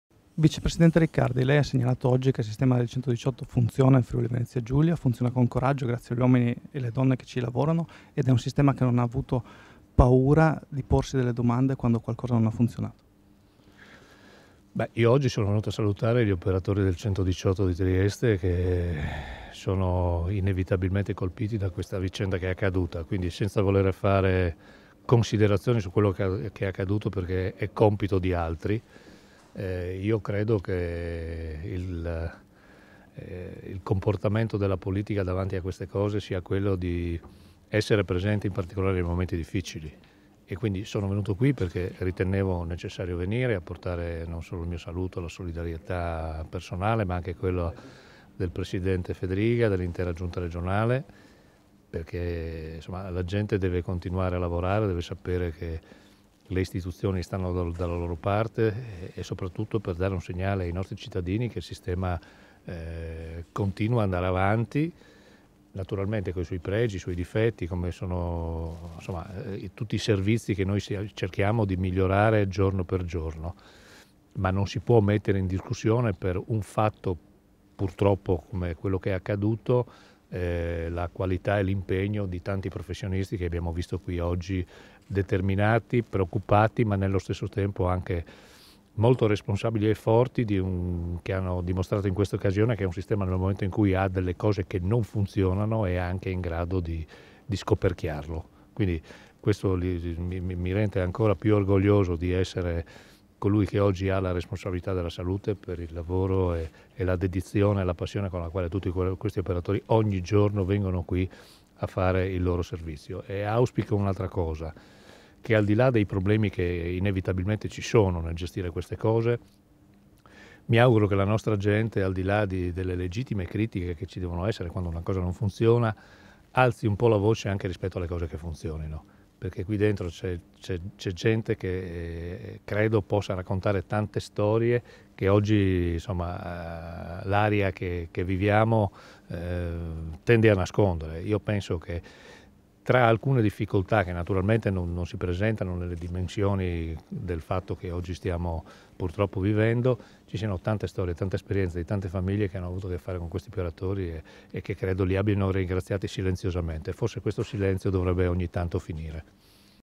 118: Riccardi, Regione a fianco professionisti con spirito di servizio
"Il servizio 118 del Friuli Venezia Giulia è qualificato, continua a fare il proprio lavoro e a dare risposte alle necessità dei cittadini. La città di Trieste e tutti coloro che ricorrono al sistema dell'emergenza-urgenza devono sapere che il 118 mette in campo operatori determinati e preparati, che non hanno esitato a segnalare situazioni anomale per tutelare i cittadini".